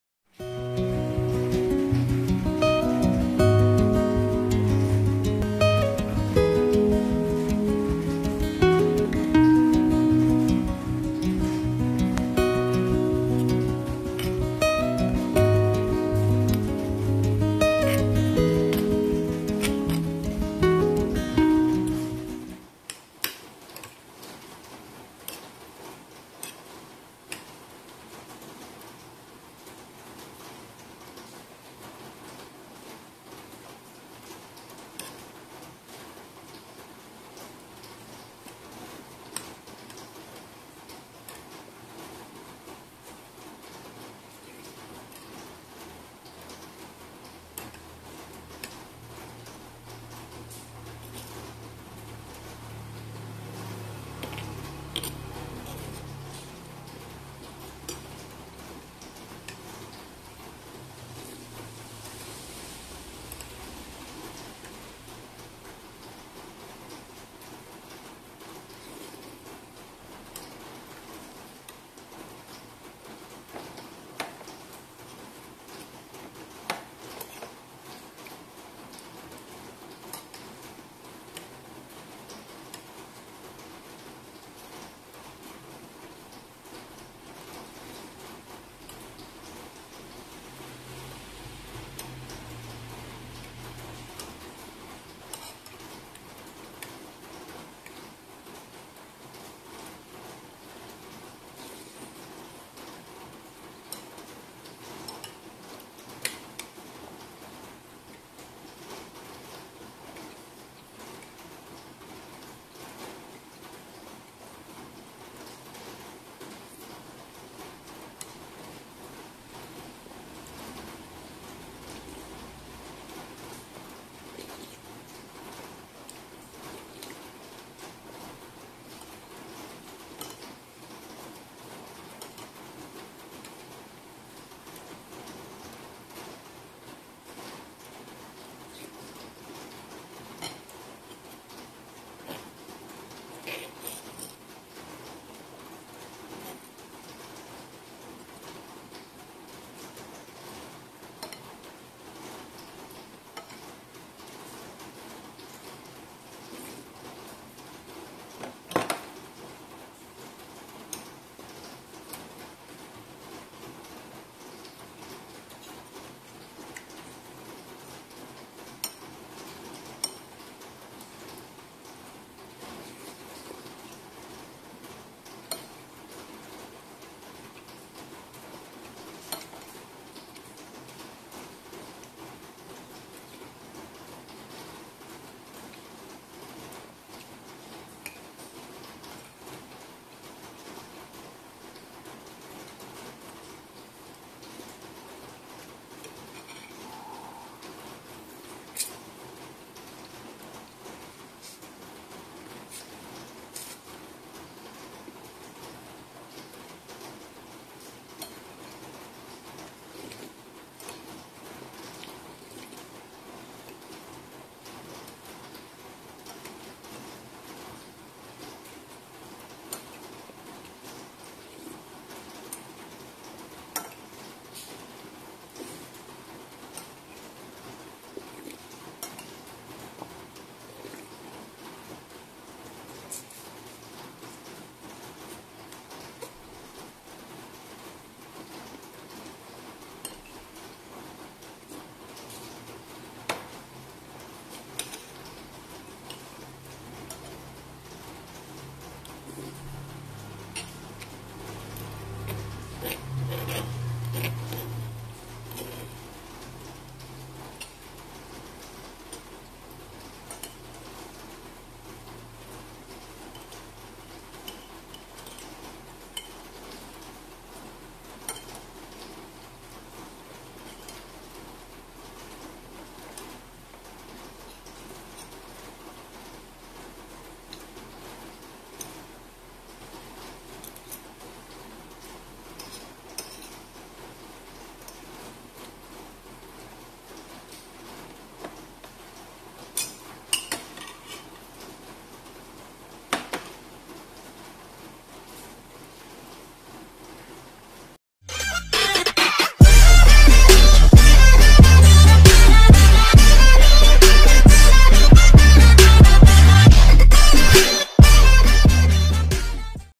ASMR mukbang sayur asem jawa telur dadar dan sambel penyet tempe SUWEGERRR 🤤
asalamuallaikum wr'wb

halo teman - teman, kali ini saya akan mukbang sayur asem telur dadar dan sambel bawang penyet tempe nih rasanya gk kalah sama masakan emak di rumah... hehe